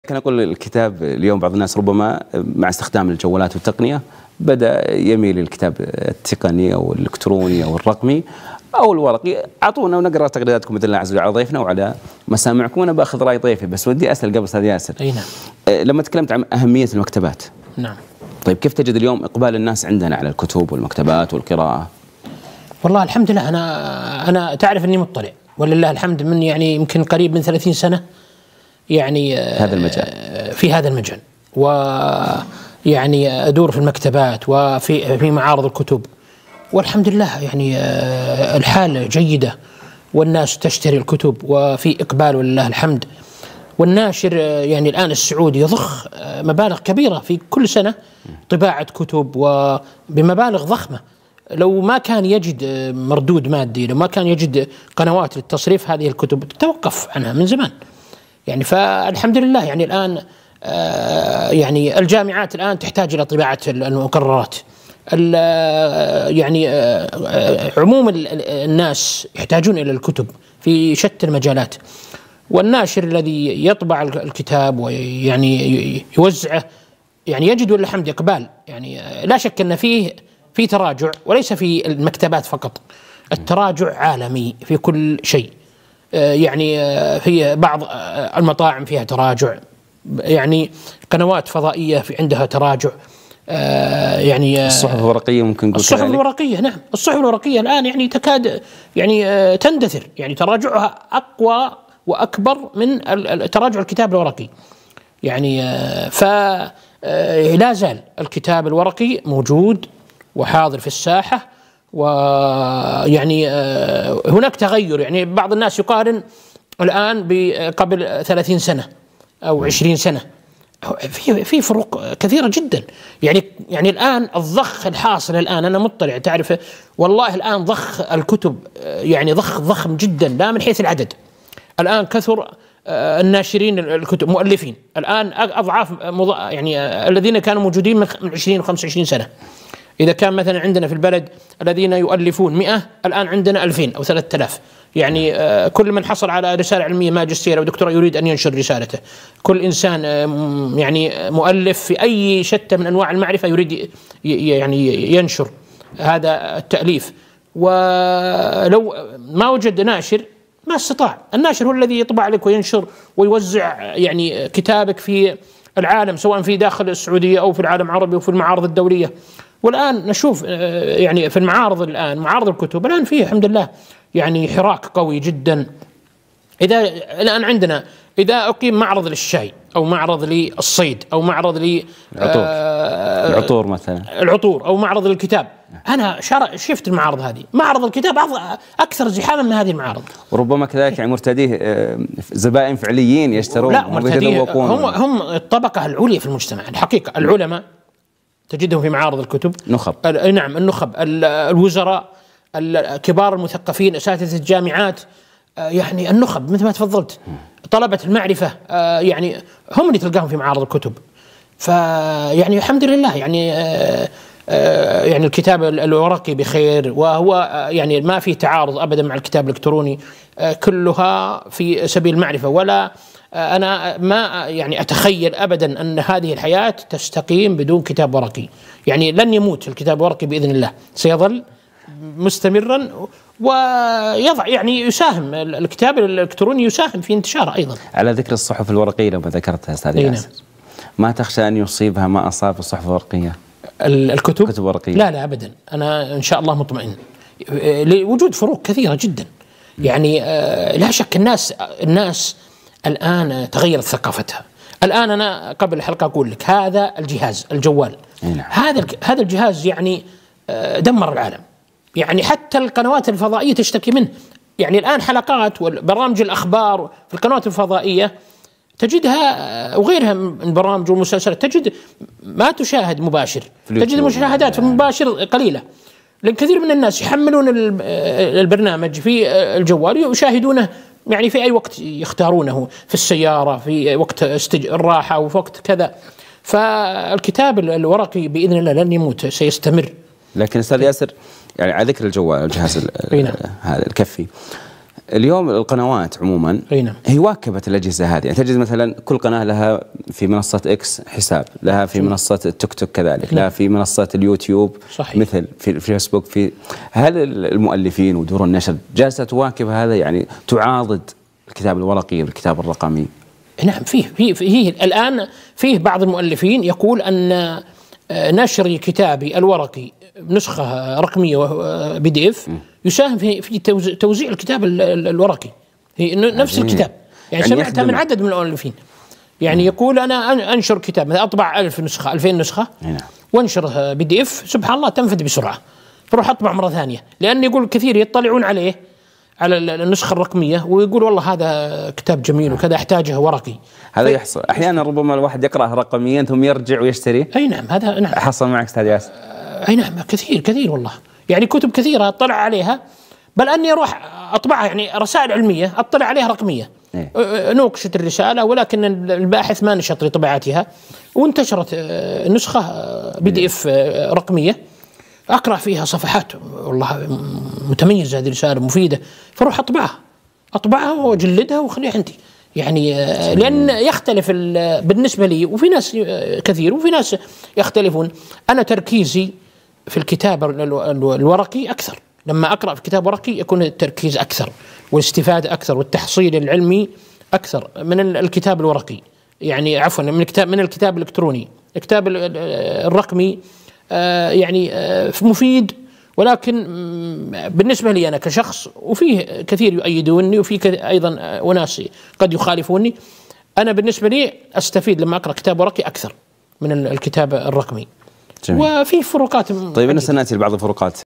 أيهما أفضل الكتاب الورقي أم الرقمي؟ | أ. ياسر الثميري
تقديم:  عبدالله الشماسي

وفي ضيافته: أ. ياسر الثميري
باحث ومهتم بالقراءة

للحديـث عن:
مستقبل الكتاب

الأحد 10:00 م
على

13 قناة آمنة لأسرتك!